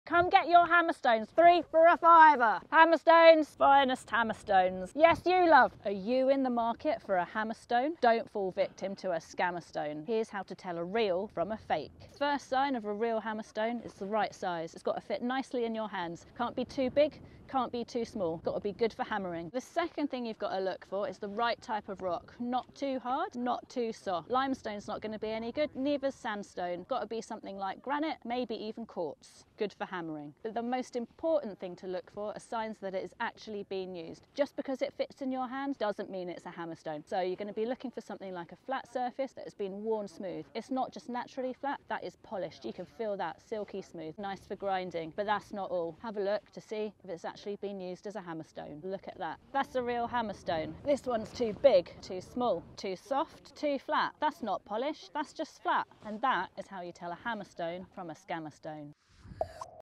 Hammerstones vs Scammerstones | Caerfai 2022
How to you tell a hammerstone from a ‘scammerstone’?

We found some incredible prehistoric archaeology on our crowdfunded dig at Caerfai Promontory Fort in Wales last year. But when most things are made of stone, how do you tell your rocks from your artefacts?

Maiya shares how to spot the real from the fake.